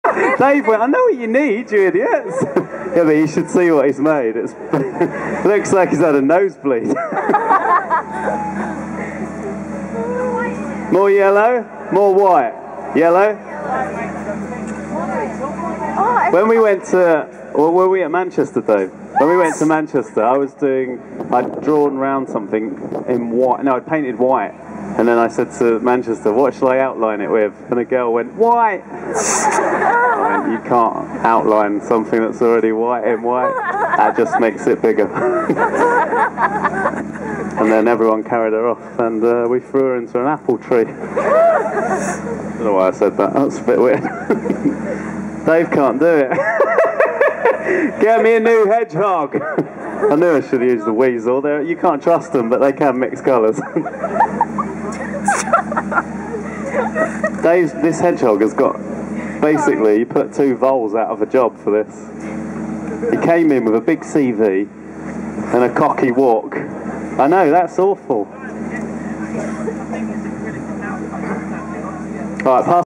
Noel Fielding in Golden Square 2011
Recorded at Noel's 2011 Scribbings of a Madcap Shambleton event in Golden Sq, London.
Noel berating Dave for not being able to mix a satisfactory orange colour... enjoy the ridiculous giggle around the 45 second mark.